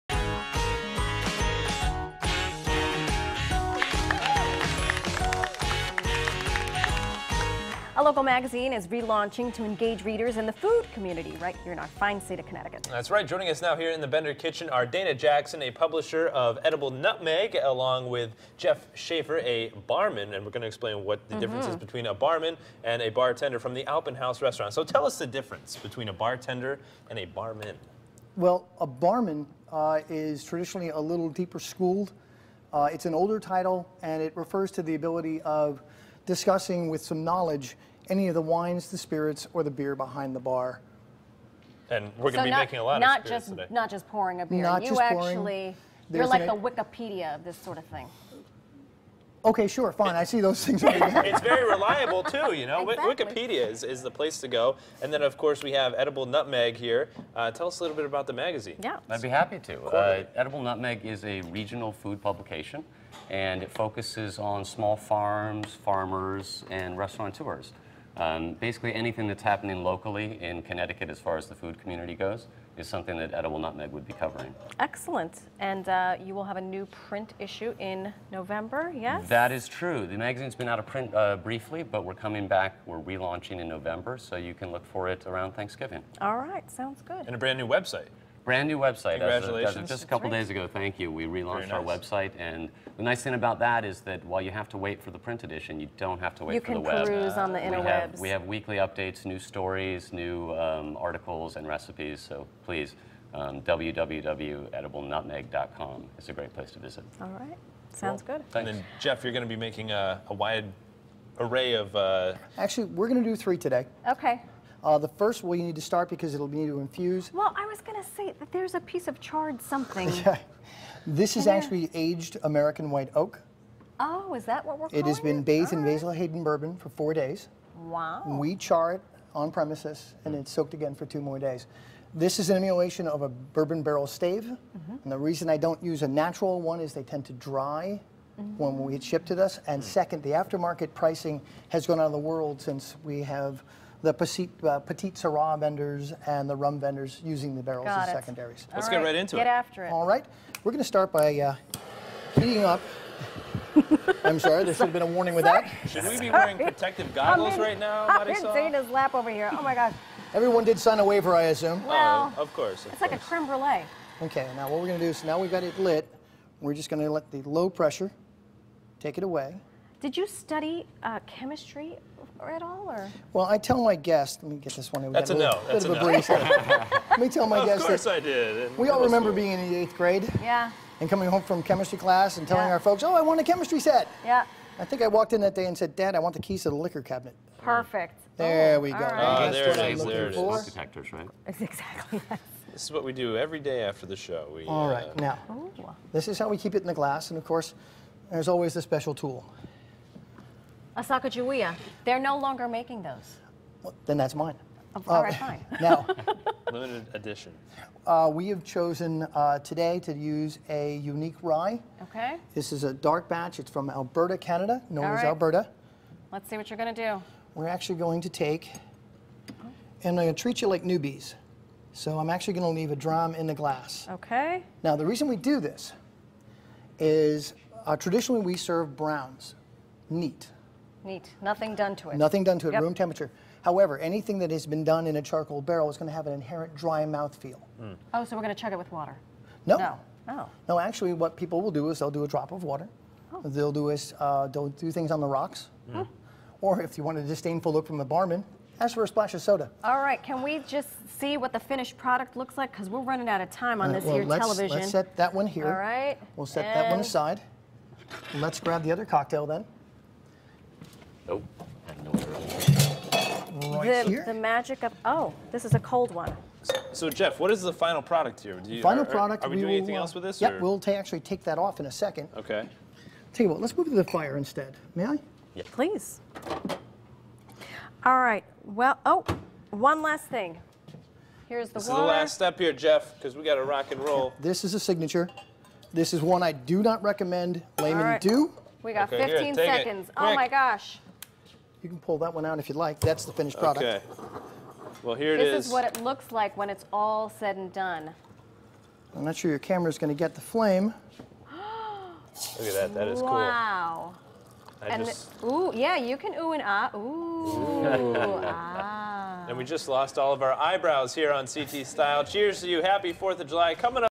Today in the Bender Kitchen: Edible Nutmeg
Ryan and Marysol learn about a new food magazine and how to make a specialty drink.